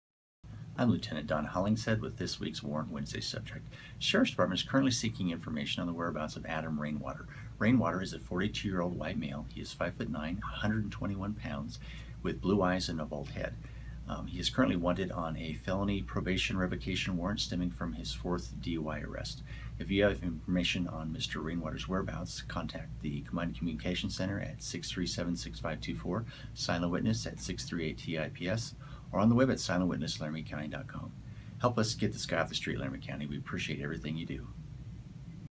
Warrant Wednesday Cheyenne Man Sought After 4th DUI Charge
[Video Description]